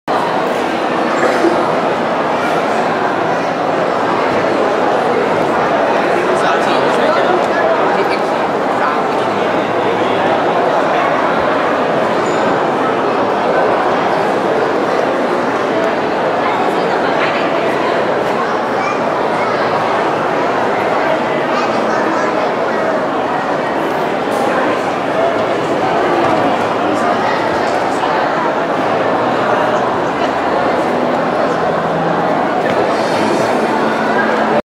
(HD) Walking in a Hong Kong Mall
Taken on the afternoon of July 18, 2011